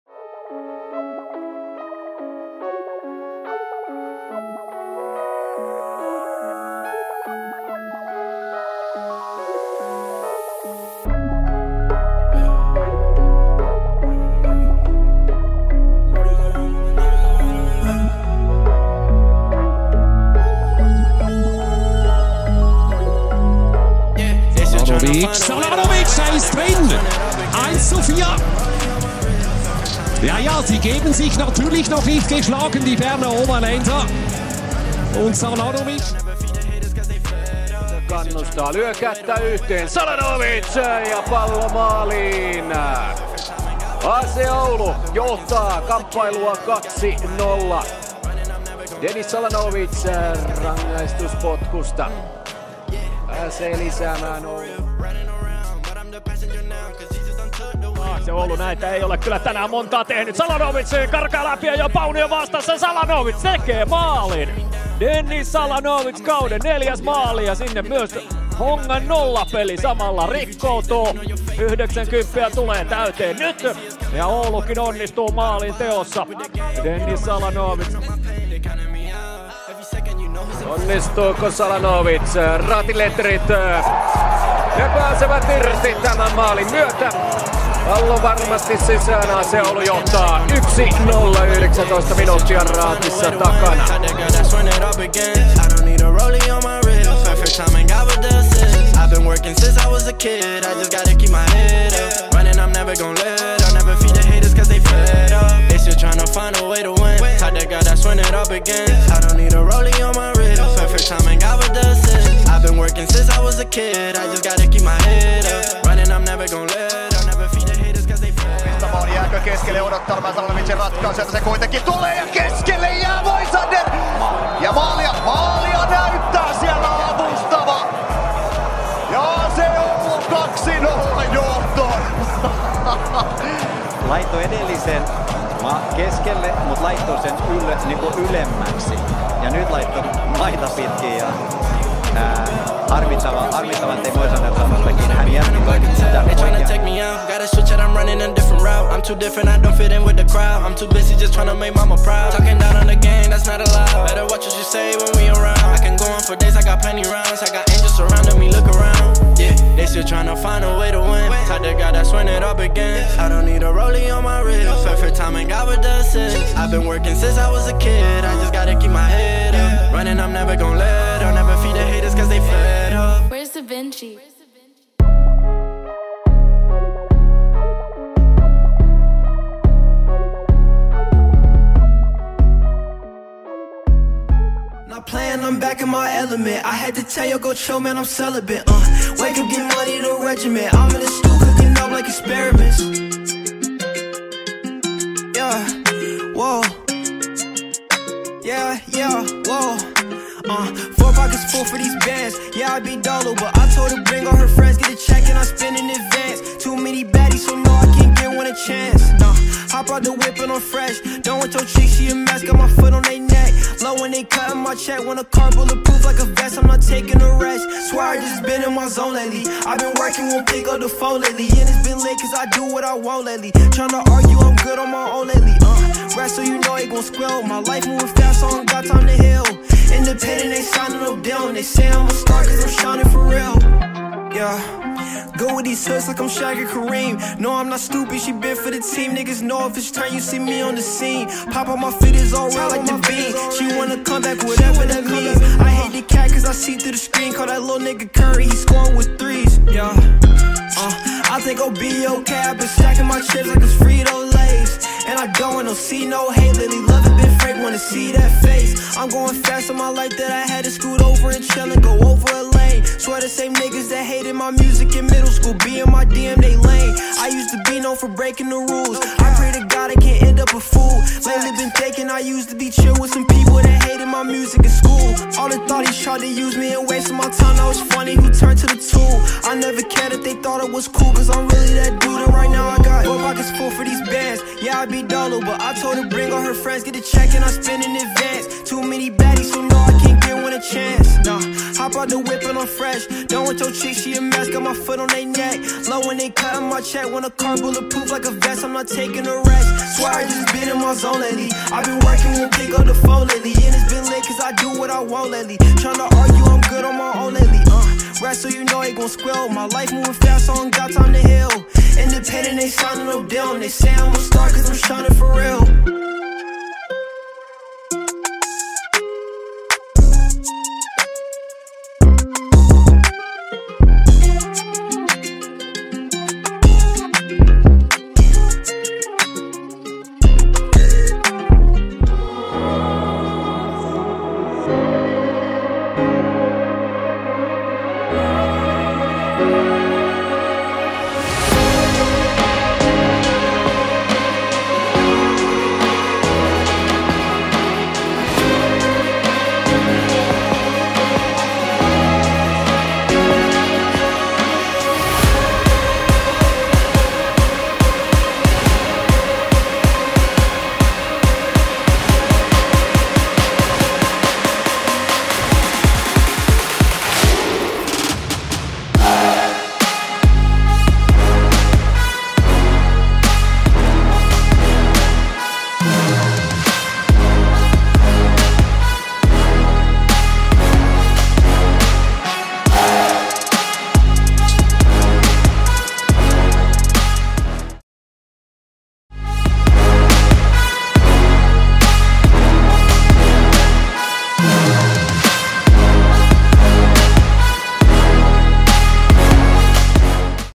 DENNIS SALANOVIC ► Best Skills, Goals & Assists (HD) 2022
🎥 Football highlights
👤 Player: Dennis Salanovic
►Dob: Feb 26, 1996
►Height: 181 CM
►Weight: 75 KG
►Position: Winger

Special credits: